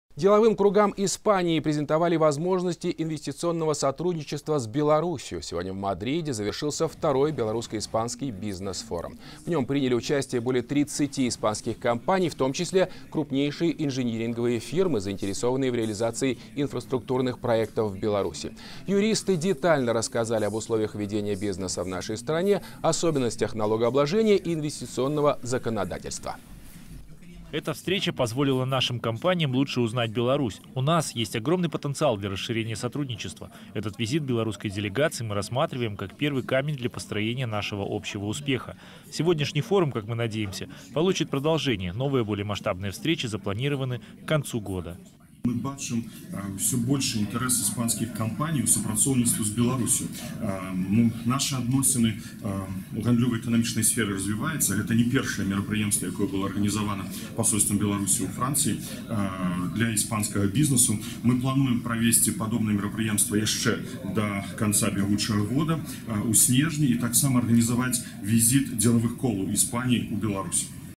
​II Белорусско-испанский бизнес-форум прошёл в Мадриде
Второй Белорусско-испанский бизнес-форум завершился в Мадриде 3 июня. В нём приняли участие более 30 испанских компаний, в том числе крупнейшие инжиниринговые фирмы, заинтересованные в реализации инфраструктурных проектов в Беларуси.

Подробности: